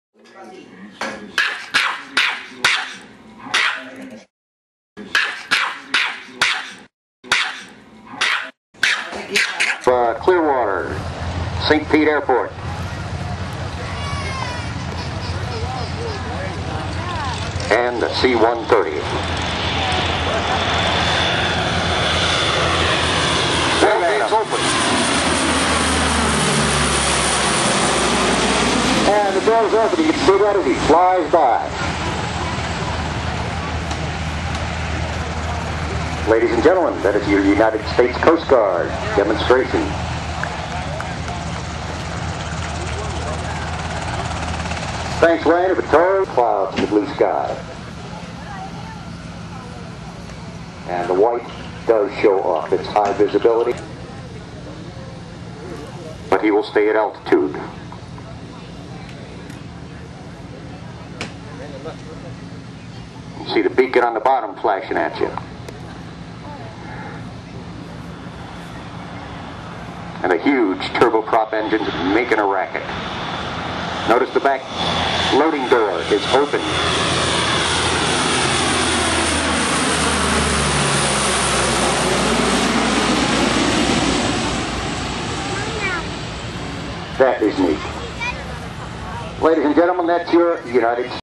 USCG C-130 Hercules Airplane Exhibition
St. Petersburg, Florida Airshow 2006. The HC-130 Hercules is a long-range surveillance and transport, fixed-wing aircraft that is used to perform search and rescue, enforcement of laws and treaties, illegal drug interdiction, marine environmental protection, military readiness, International Ice Patrol missions, as well as cargo and personnel transport.